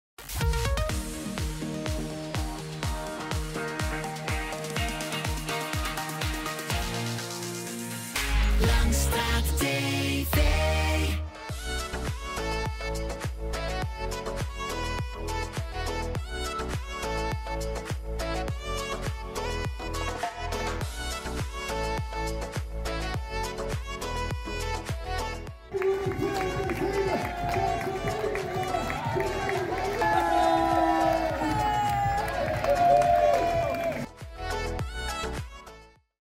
Versieringen en gejuich: Roparun een succes
De Roparun kwam op 5 juni door De Langstraat. Het evenement was ondanks het weer een groot succes.